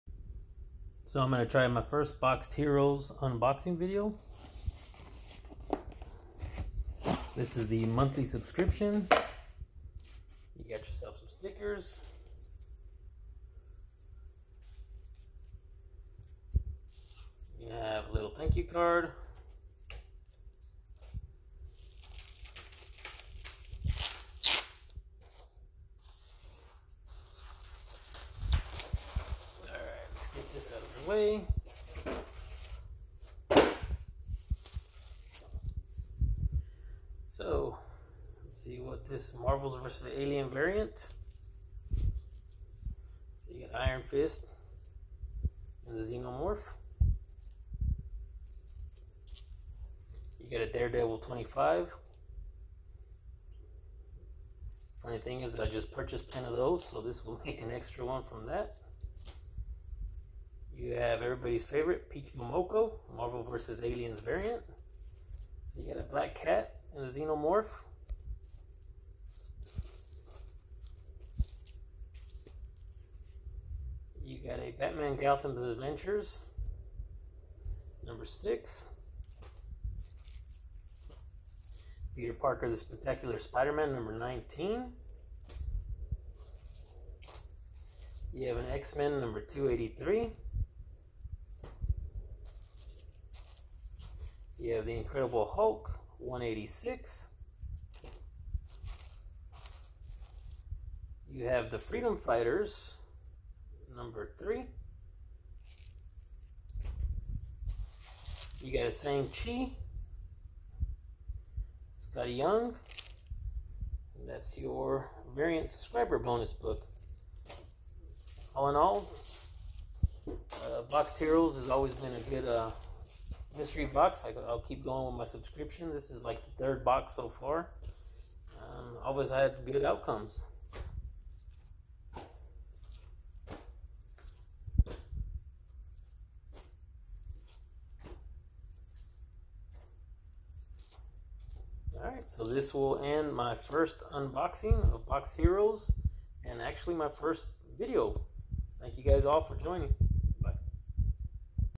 Boxed heroes monthly subscription unboxing
Unboxing a boxed heroes mystery box. First ever video